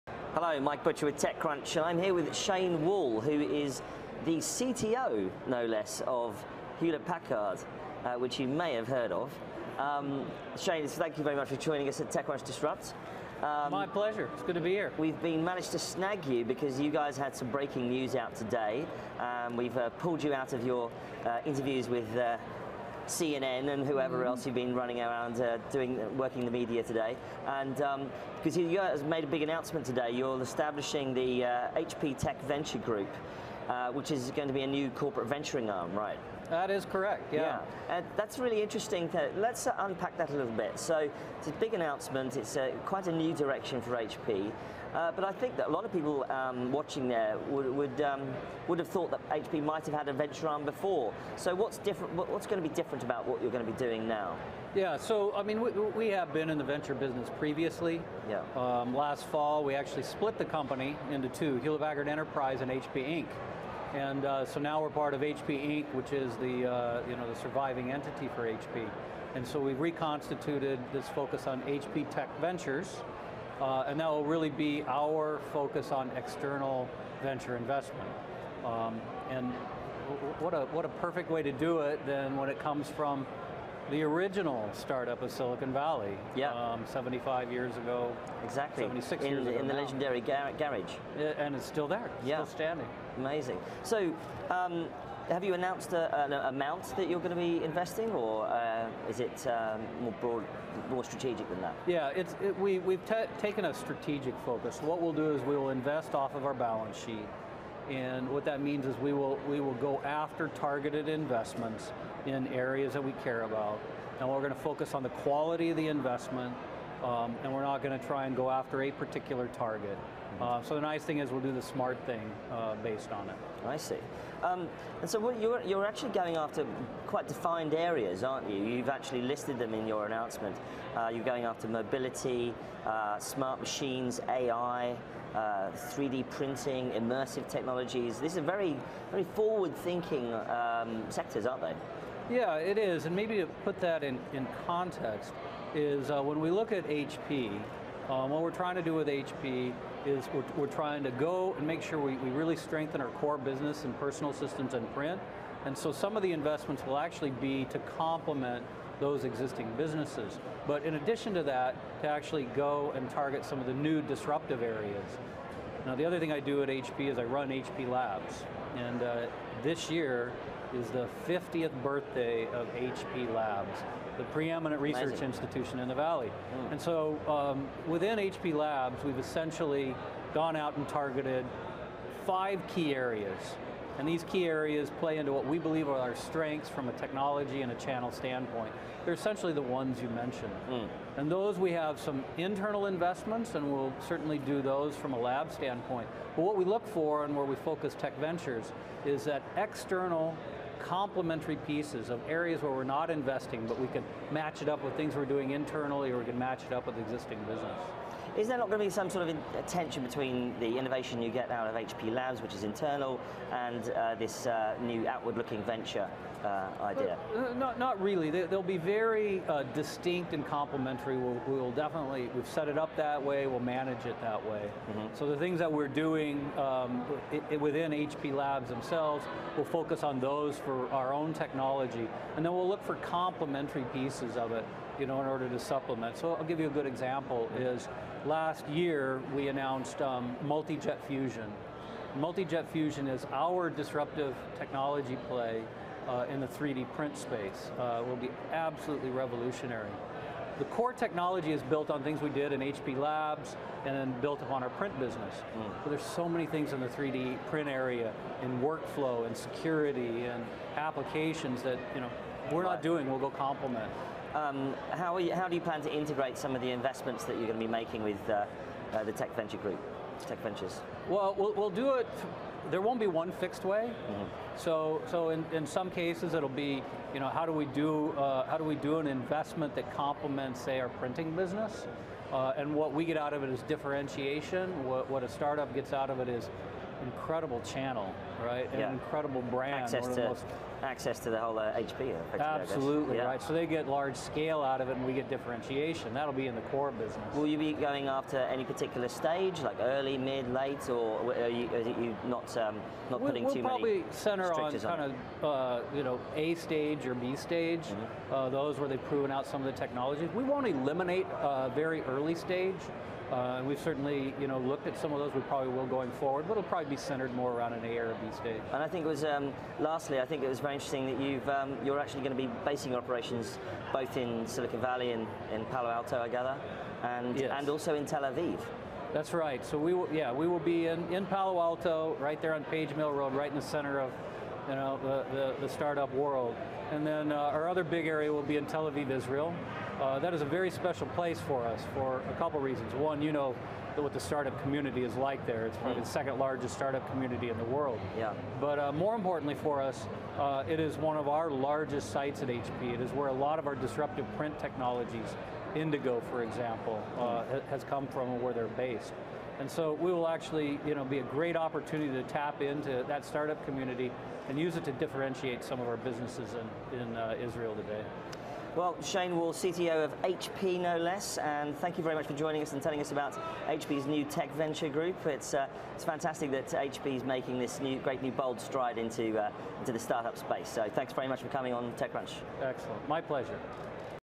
HP Launches Tech Venture Group
HP Inc. CTO Shane Wall talks to Mike Butcher about today’s launch of the HP Tech Venture Group. Walls says they’ll focus on mobility, AI, 3D printing, immersive technology and other disruptive areas.